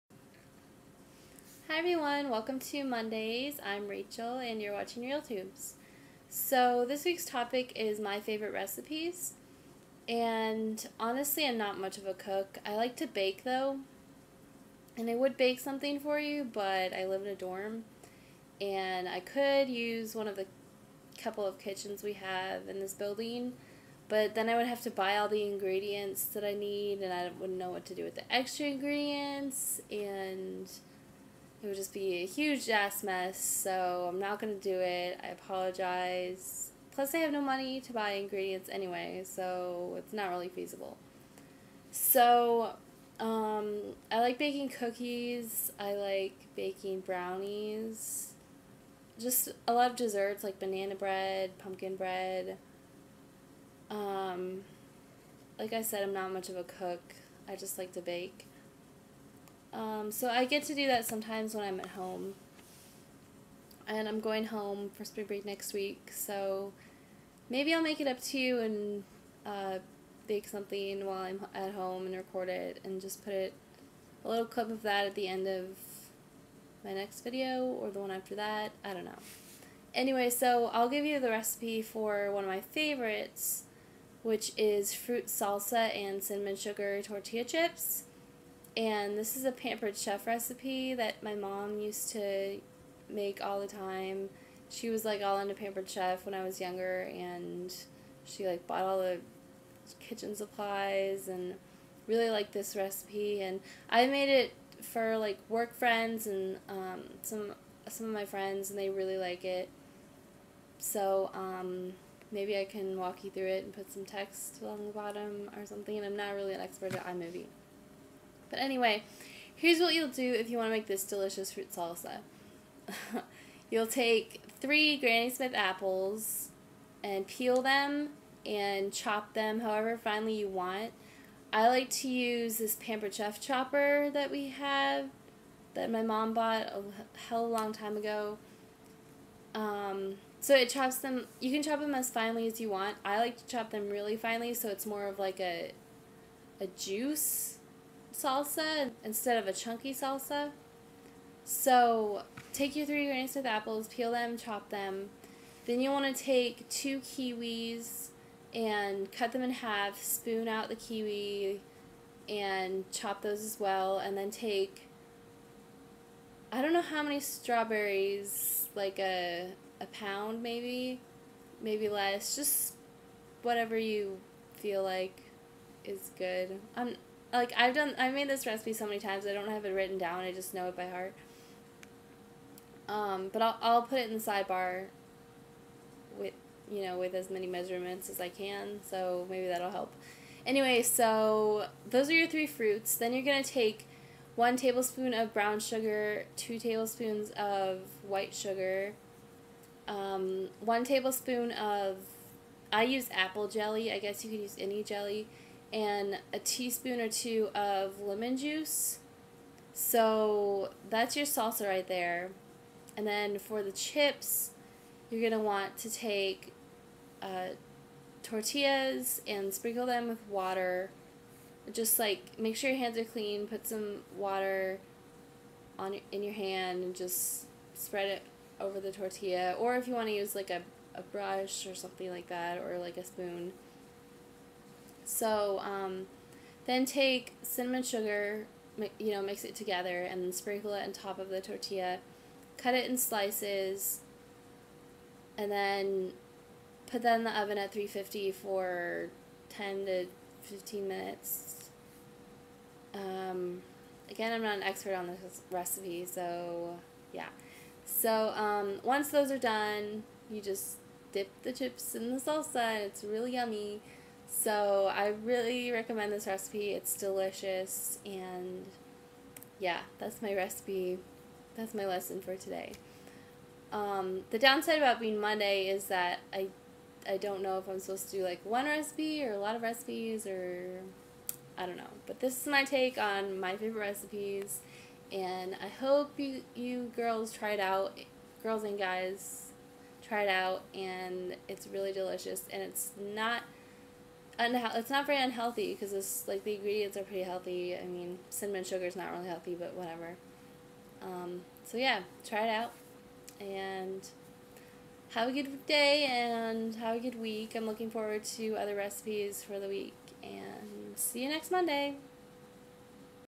[Monday 3/16] My Favorite Recipies
So this week I talked about one of my favorite recipes, fruit salsa with cinnamon sugar tortilla chips:

Chop (however finely you prefer):
3 peeled Granny Smith apples
2 kiwis
1 quart? strawberries

Mix in:
1 tbsp. brown sugar
2 tbsp. white (granulated) sugar
2 tsp. lemon juice
1 tbsp. apple (or other) jelly

Cinnamon sugar tortilla chips:

Wet tortillas with water (or I suppose you could use butter-based cooking spray) and sprinkle with a cinnamon-sugar mixture. Bake at 350 degrees for 10-15 minutes.